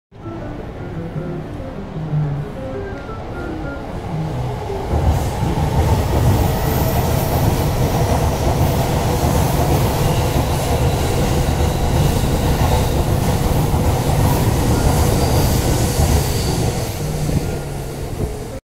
354.万世橋付近を走行する中央線E233系
2022年5月23日18時48分頃
神田駅から御茶ノ水駅に向かう中央線です。